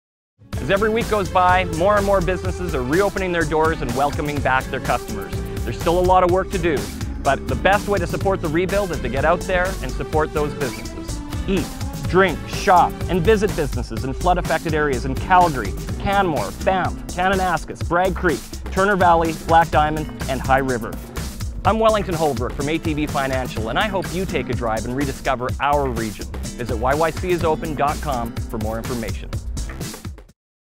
Rediscover our Region - High River
Hundreds of businesses have suffered significant losses of revenue due to the flood. Show your supporting by visiting the flood-affected regions: High River, Bragg Creek, Turner Valley, Black Diamond, Banff, Canmore and Kananaskis